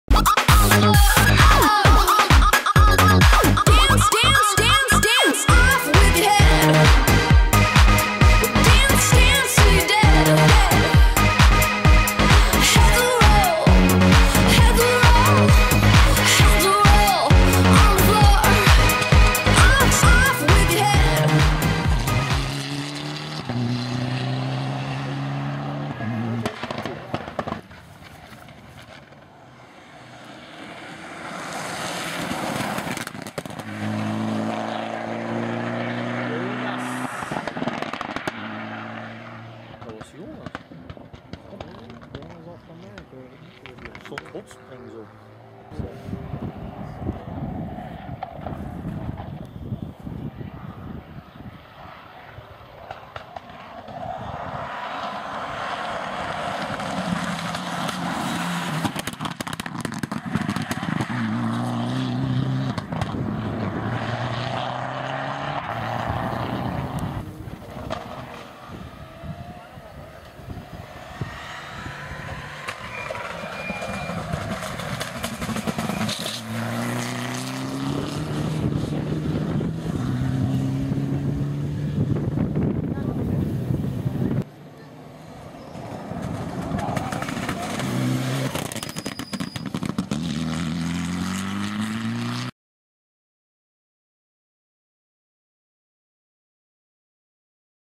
GEKO Ypres Rally 2011 - Jonas Langenakens
Jonas Langenakens en Nico Beernaert in de GEKO Ypres Rally 2011